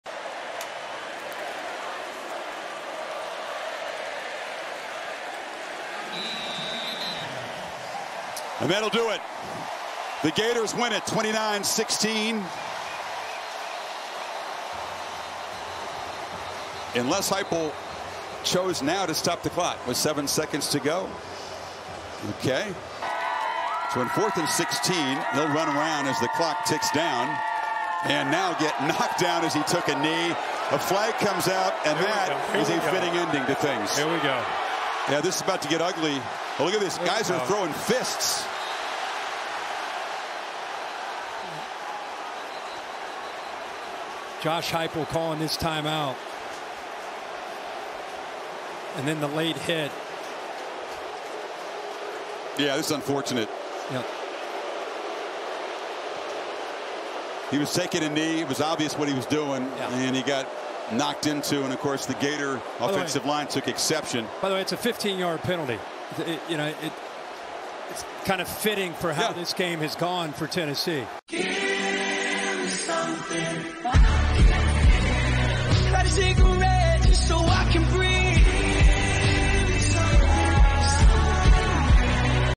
Florida's bench clears after Graham Mertz takes illegal hit in final play vs. Tennessee 👀
As the Florida Gators wrap up their 29-16 win vs. the No. 11 Tennessee Volunteers players get into a heated exchange where punches are thrown.